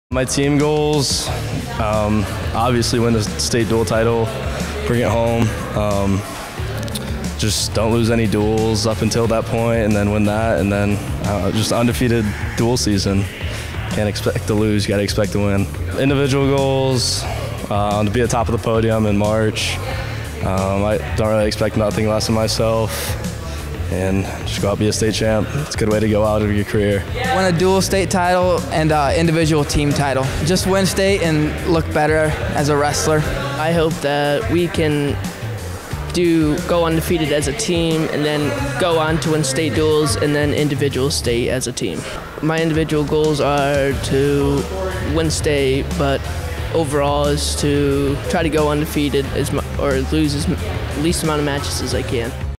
2019 EDISON CHARGERS WRESTLING
We had a chance to talk to a few of the members of the Edison Chargers wrestling team the other day about some of their personal goals and their team expectations 🤼‍♂️